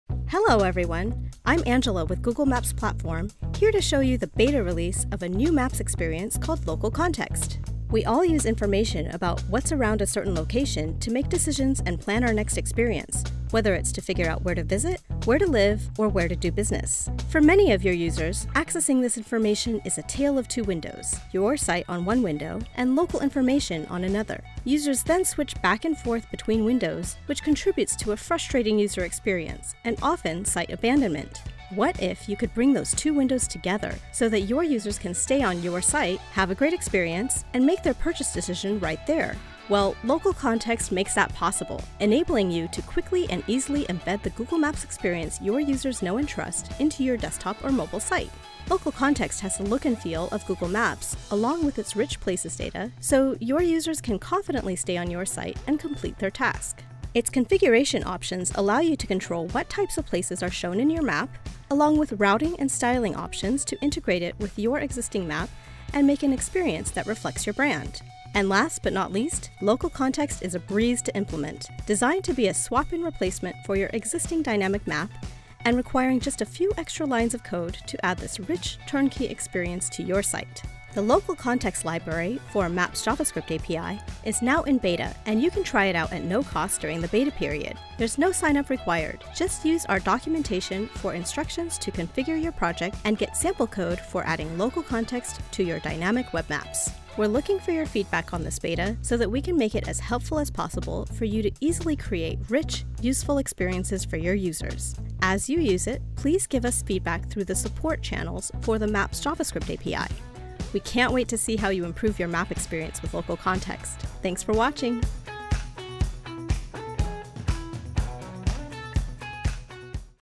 Announcing: Local Context beta for Maps JavaScript API
Help users make quicker decisions with Local Context, a new Maps experience available in beta through the Maps JavaScript API.